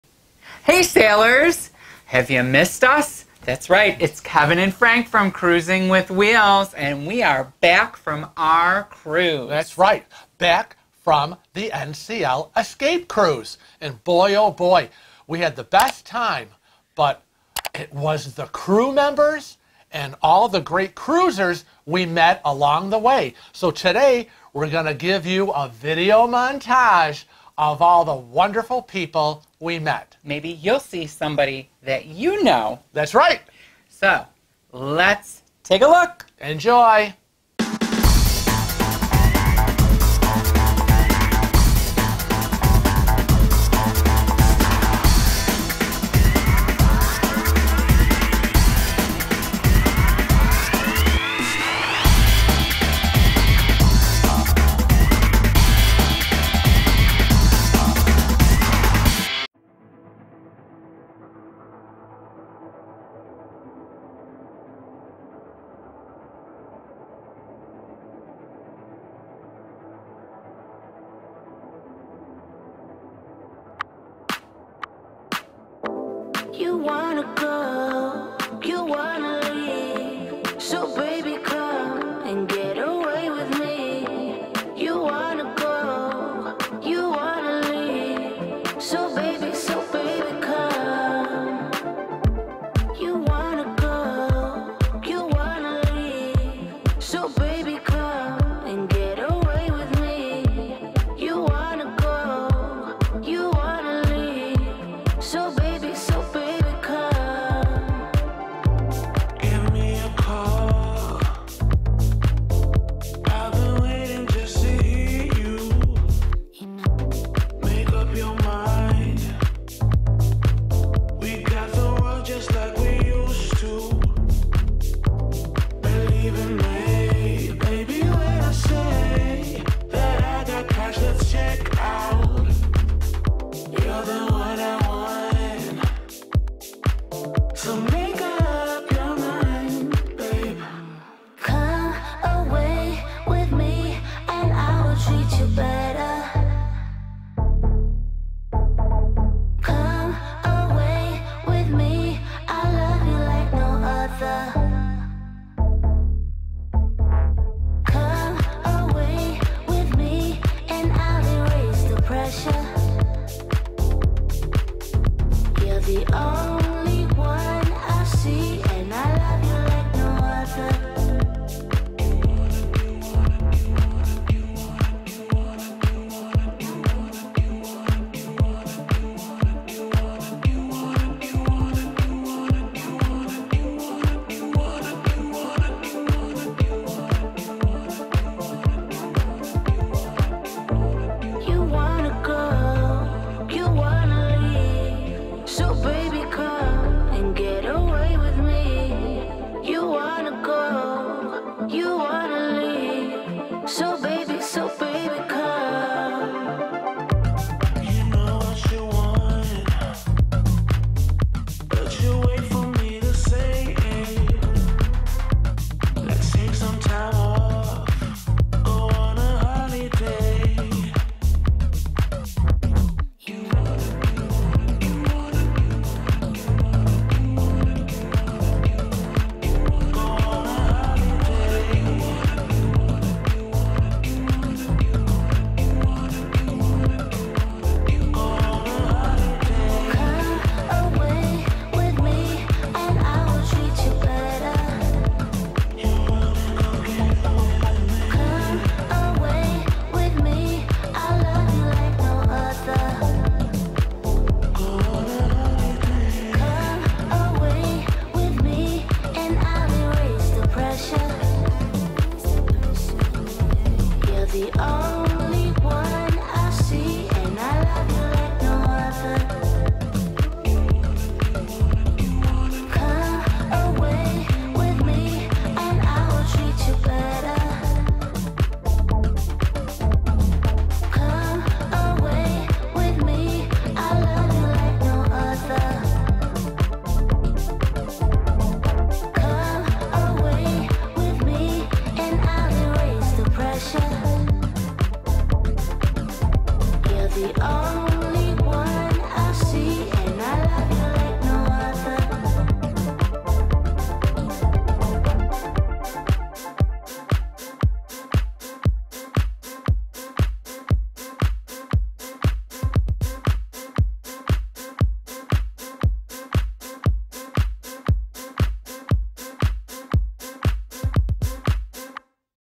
People We Met NCL Escape
In this video ) we share with you all the crew and subscribers we met on the NCL Escape!!  Maybe you will see someone you recognize!!

Useful Links -

Cruising With Wheels Social Media Links

Mailing address -